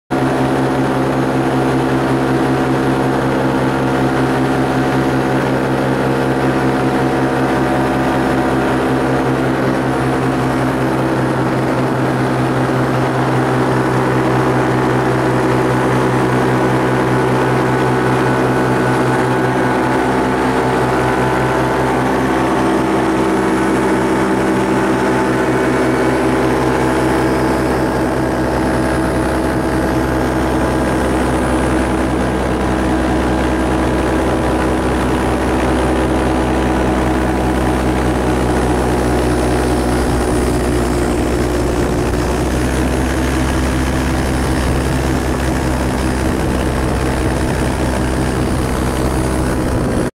feuille d'automne vrille.mpg
petit 360 engagé, filmé par Julien Grenon au dessus de Shawinigan
5 oct 2010